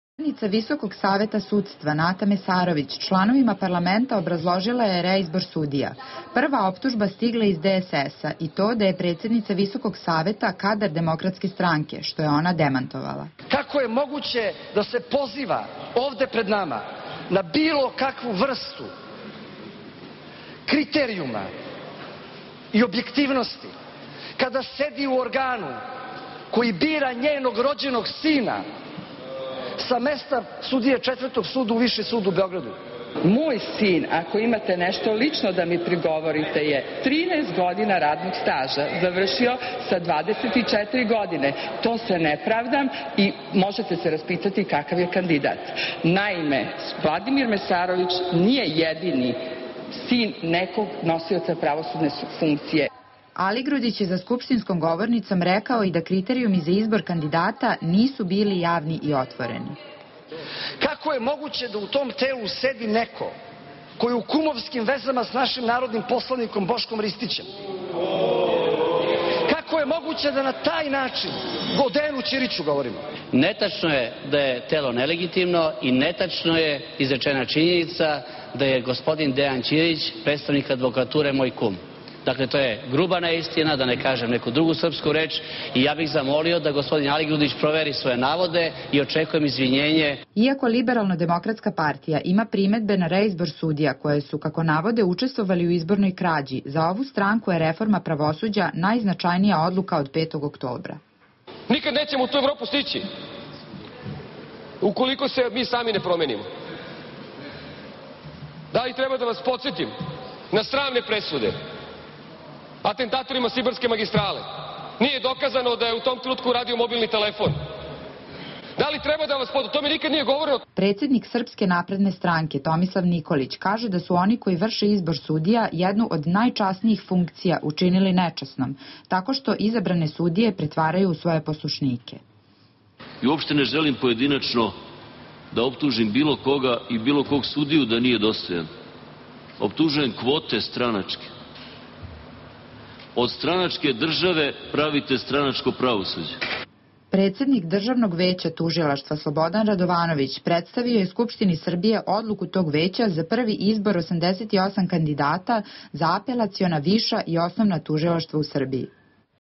Opozicija kritikuje izbor sudija
Poslanici vladajuće
koalicije podržavaju izbor sudija i ističu važnost reforme pravosuđa, dok
veći deo opozicije sumnja da je to izbor politički podobnih.